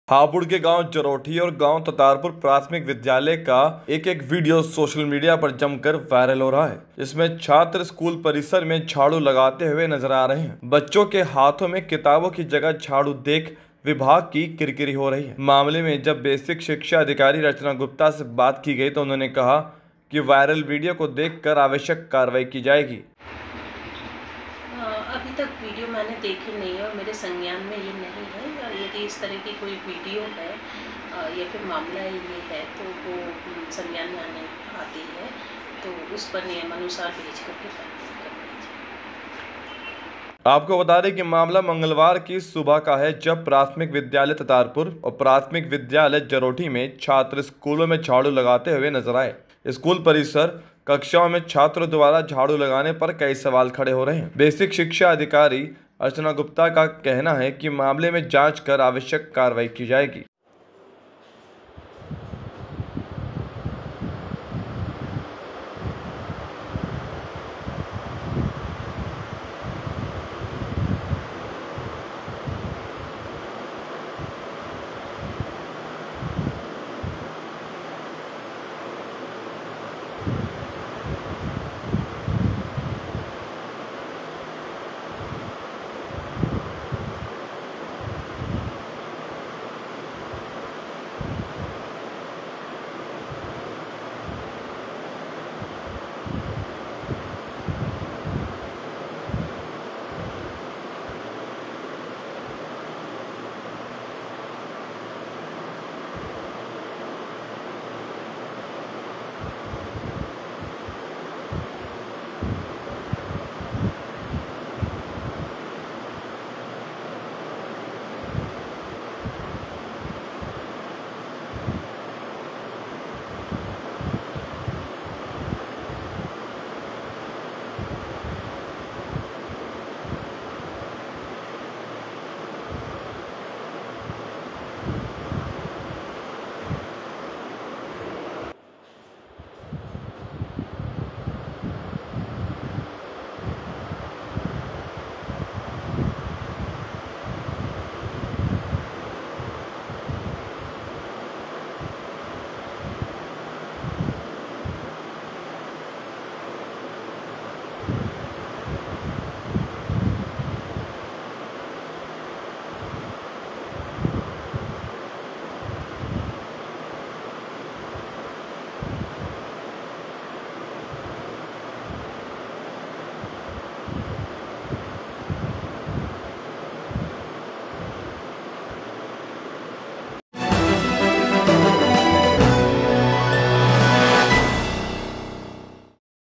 स्कूल में झाड़ू लगाते छात्रों का वीडियो वायरल
हापुड़ के गांव जरोठी और गांव ततारपुर प्राथमिक विद्यालय का एक वीडियो सोशल मीडिया पर वायरल हो रहा है जिसमें छात्र स्कूल परिसर में झाड़ू लगाते हुए नजर आ रहे हैं। बच्चों के हाथों में किताबों की जगह झाड़ू देख विभाग की किरकिरी हो रही है। मामले में जब बेसिक शिक्षा अधिकारी अर्चना गुप्ता से बात की गई तो उन्होंने कहा वायरल वीडियो को देख कर आवश्यक कार्रवाई की जाएगी।
आपको बता दें कि मामला मंगलवार की सुबह का है जब प्राथमिक विद्यालय ततारपुर तथा प्राथमिक विद्यालय जरोठी में छात्र स्कूलों में झाड़ू लगाते हुए नजर आए। स्कूल परिसर, कक्षाओं में छात्रों द्वारा झाड़ू लगाने पर कई सवाल खड़े हो रहे हैं। बेसिक शिक्षा अधिकारी अर्चना गुप्ता का कहना है कि मामले में जांच कर आवश्यक कार्रवाई की जाएगी।